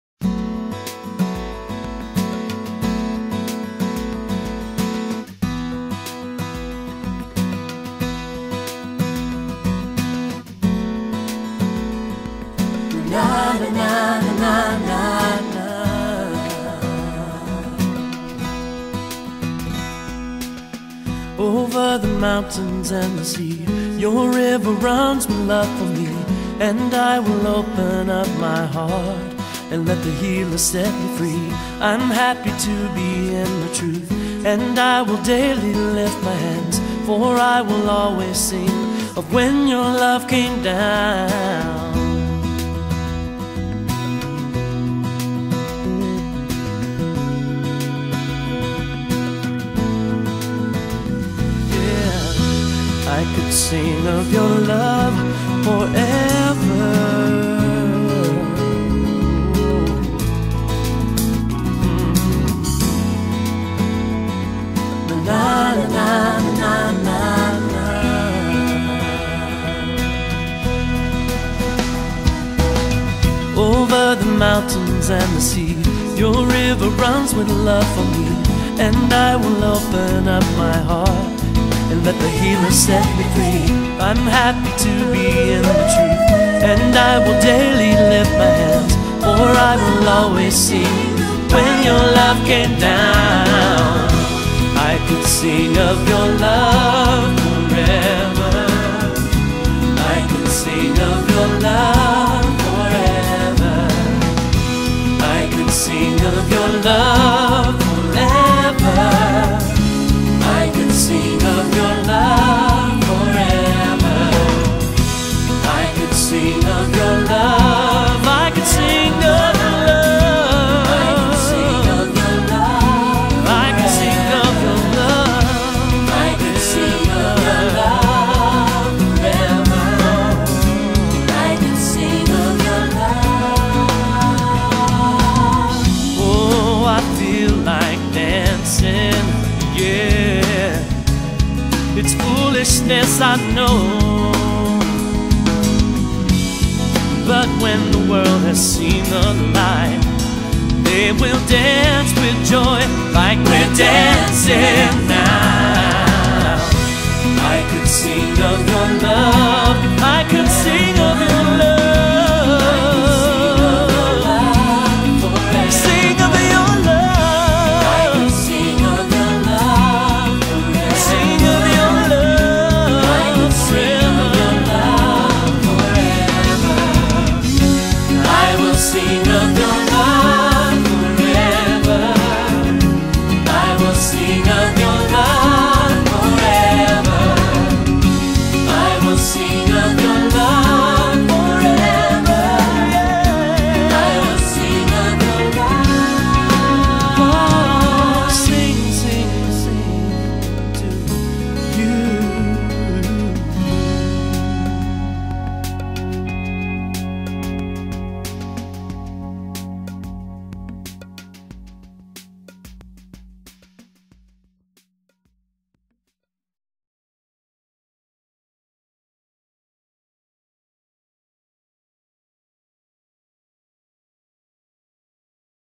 Shiloh Mini Concert 1998 (Straight from the Heart)
Shiloh Christian Fellowship Inc. presents -  Straight from the Heart - A mini Musical Concert ( Shilohites Original Compositions) last 4th of October 1998 @ Yulo Street, Iloilo City, Philippines.